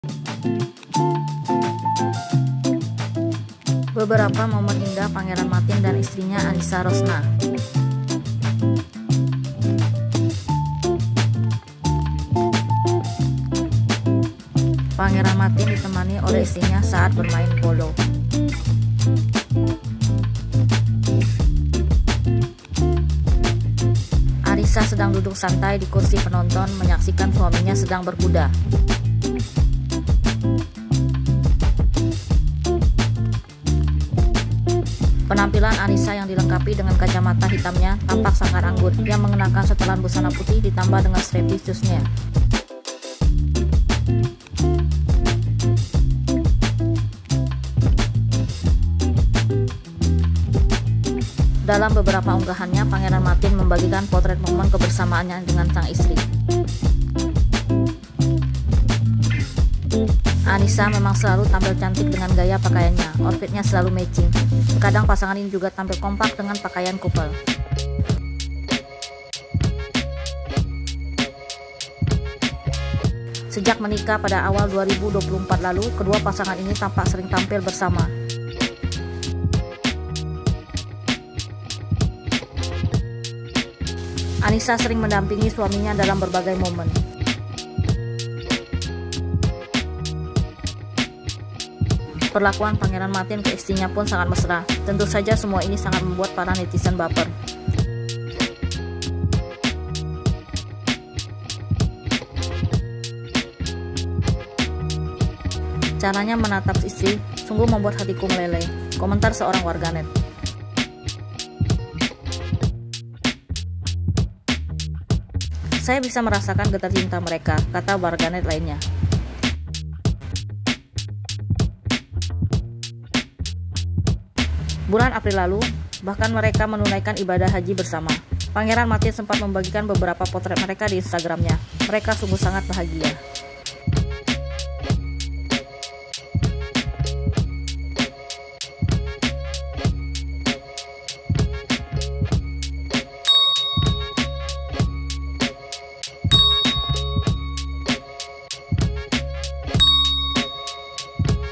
A moment of togetherness between Prince Mateen and his wife Anisha Rosnah
Since getting married, this young couple has always been together. Anisha often accompanies her husband to play polo and horse riding.